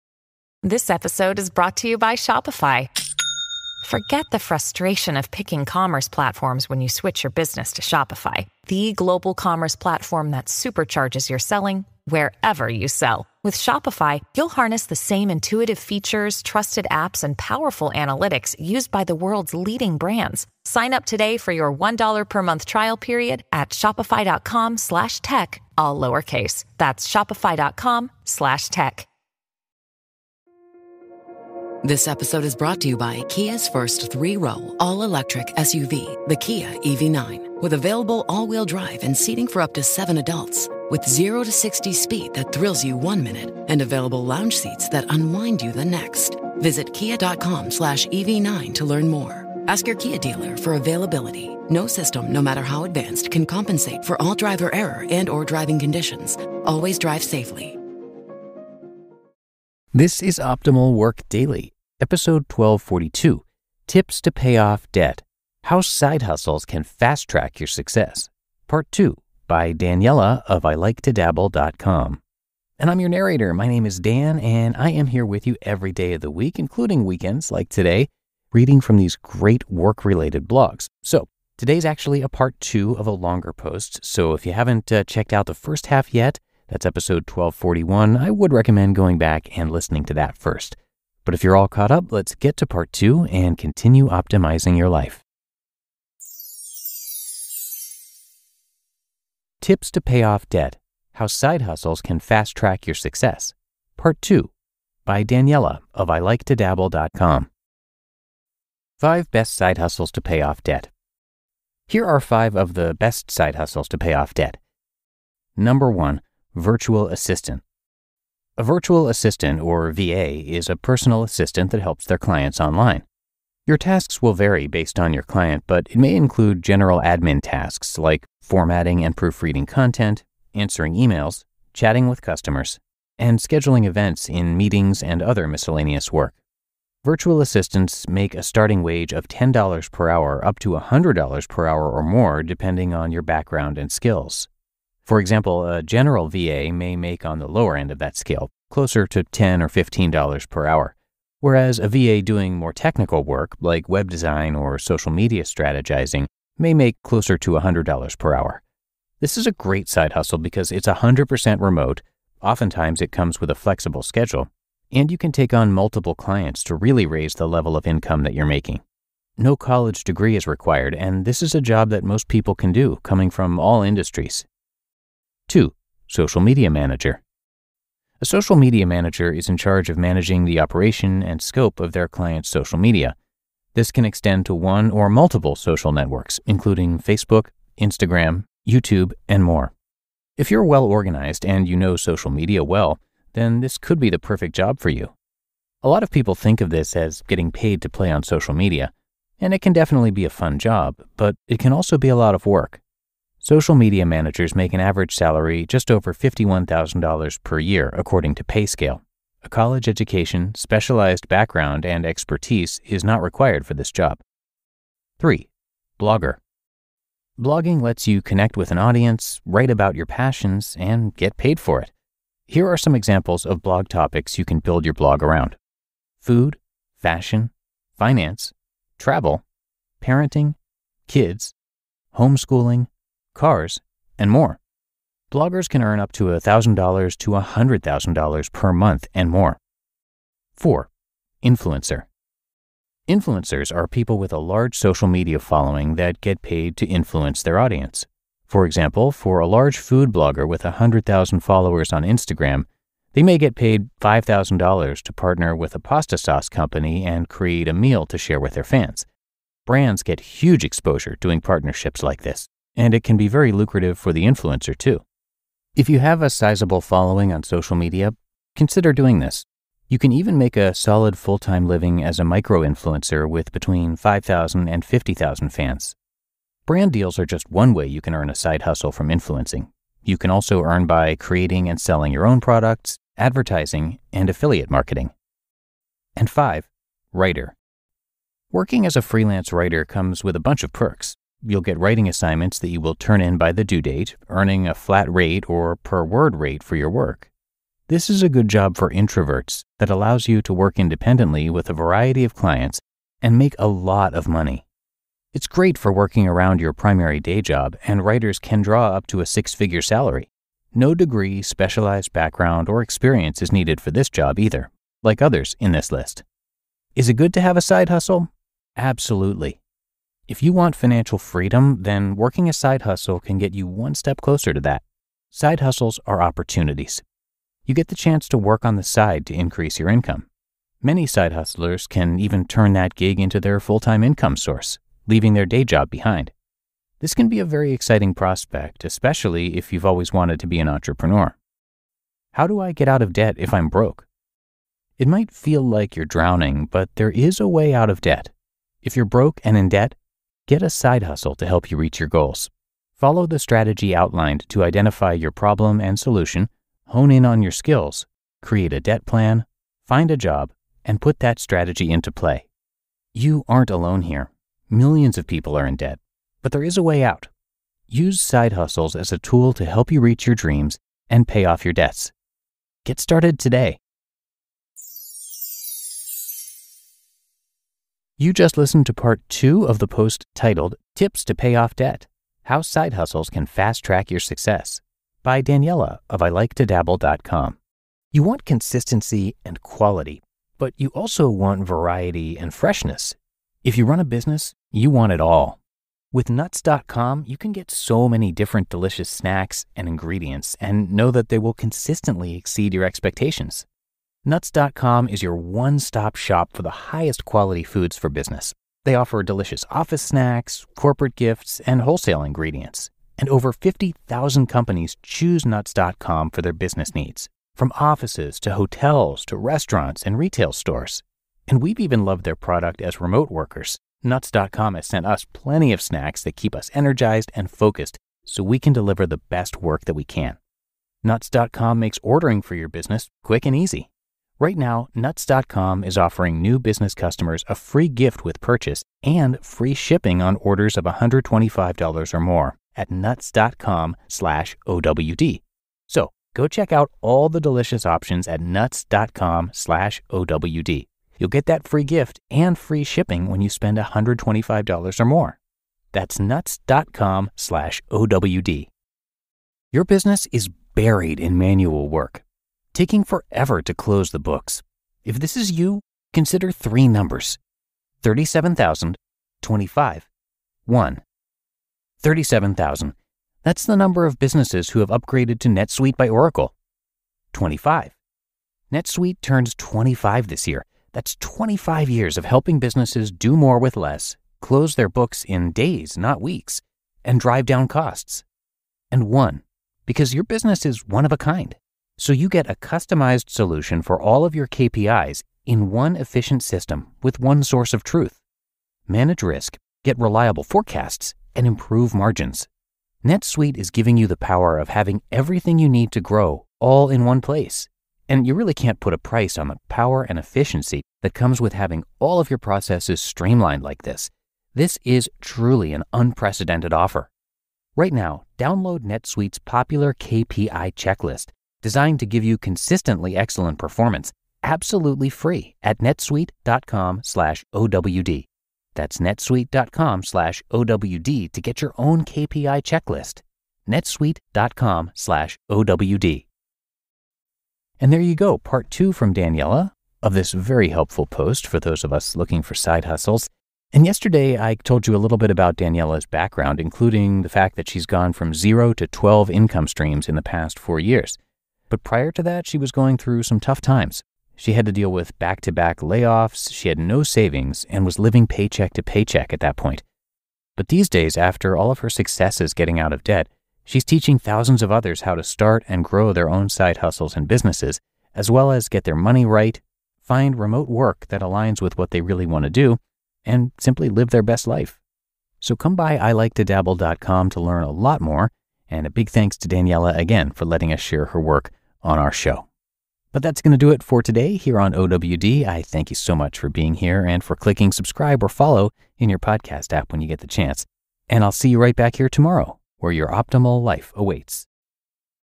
1242: [Part 2] Tips to Pay Off Debt: How Side Hustles Can Fast Track Your Success by Daniella of...
Episode 1242:
Daniella from I Like to Dabble.com offers transformative advice on leveraging side hustles to accelerate debt repayment in "Tips to Pay Off Debt: How Side Hustles Can Fast Track Your Success - Part 2." She highlights five lucrative side gigs, from virtual assisting to freelance writing, emphasizing the potential for financial freedom and the possibility of transitioning side hustles into full-time careers. Daniella's personal journey from financial instability to creating multiple income streams underscores the power of side hustles in achieving financial goals.
Quotes to ponder:
"If you want financial freedom then working a side hustle can get you one step closer to that."
"Use side hustles as a tool to help you reach your dreams and pay off your debts."